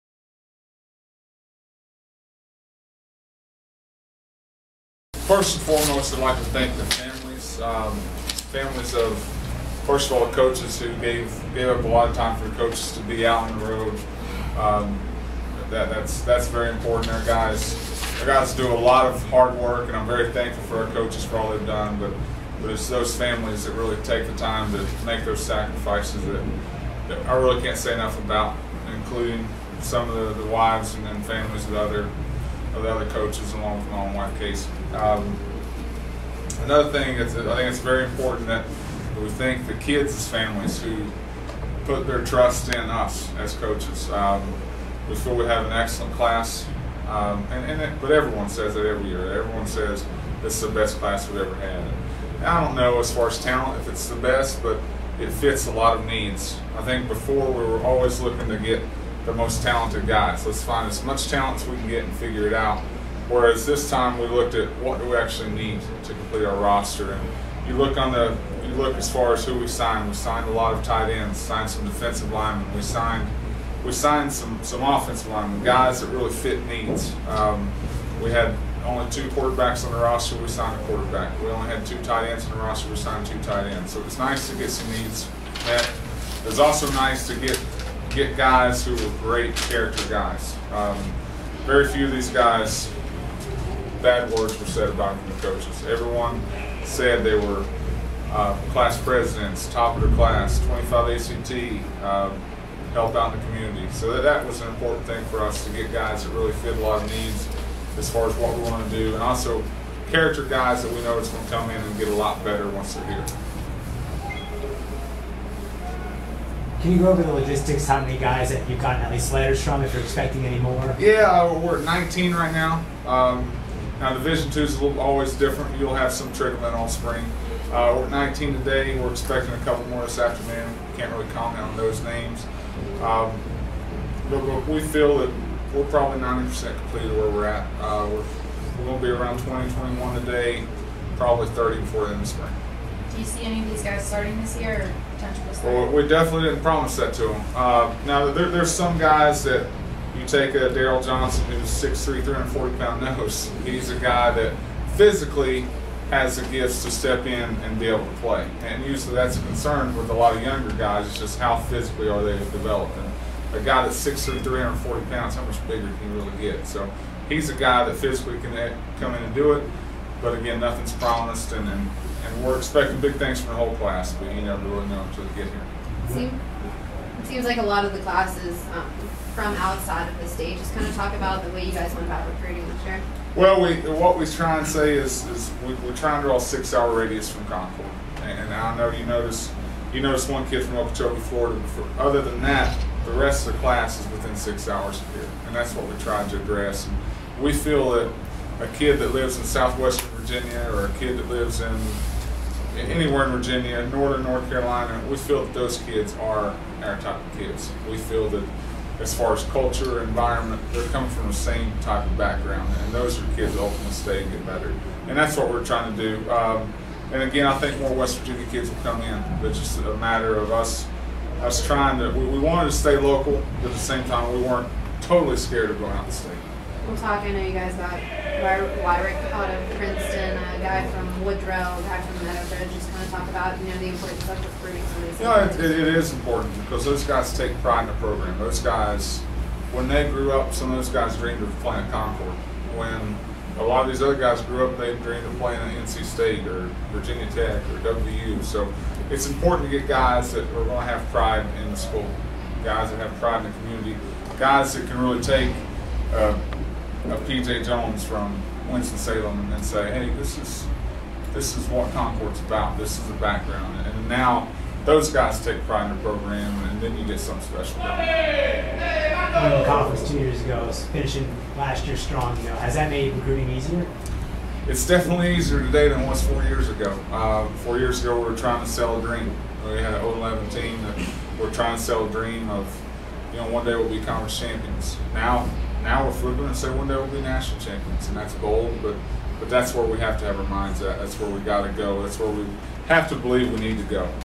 Concord University Football 2013 Signing Day Press Conference
Concord University Football 2013 Signing Day Press Conference with Head Coach Garin Justice